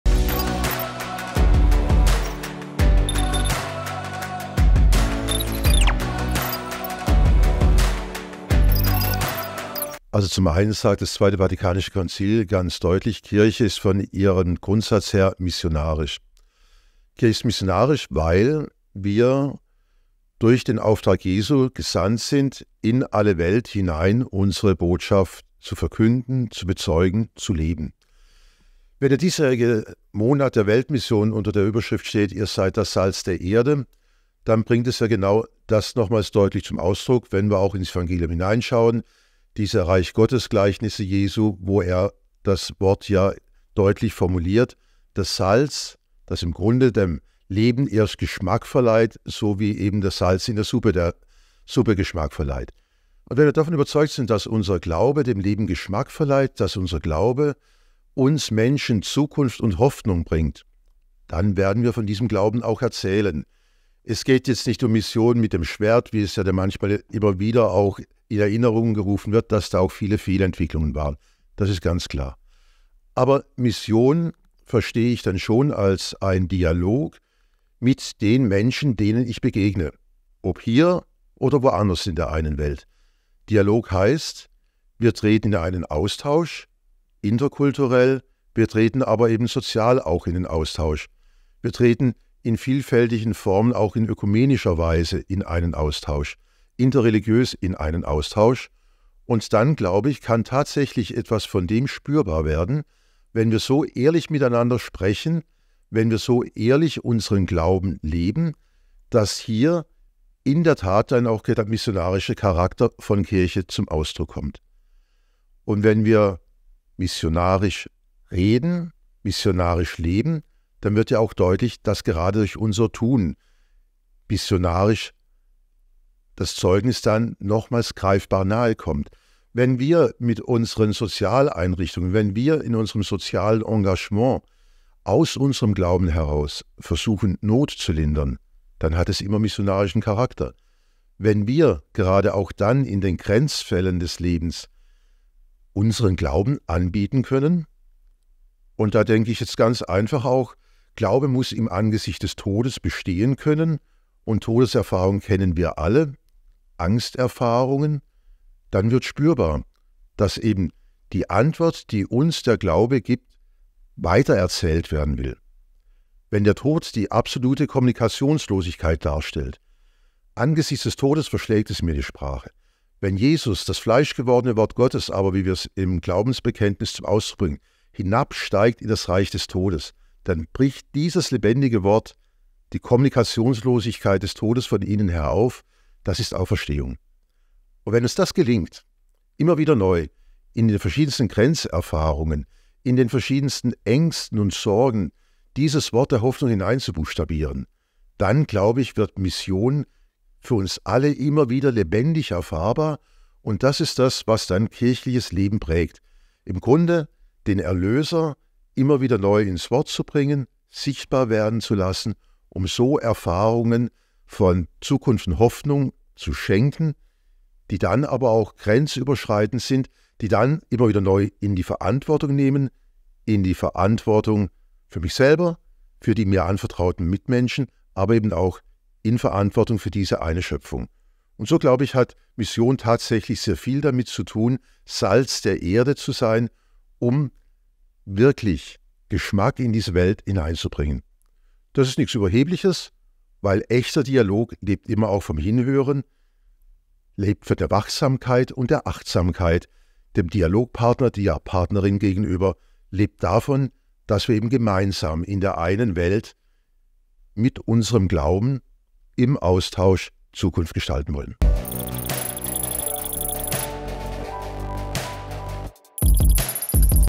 Weihbischof Dr. Peter Birkhofer - Missionszeugnis
Im Missionsmonat Oktober geben Bischöfe, Geistliche und Jugendliche bei uns Zeugnis davon, warum sie ein Missionar sind und was Mission für sie persönlich bedeutet. Heute seht ihr das Zeugnis von Weihbischof Dr. Peter Birkhofer aus dem Bistum Freiburg. Alles rund um den Missionsmonat bei radio horeb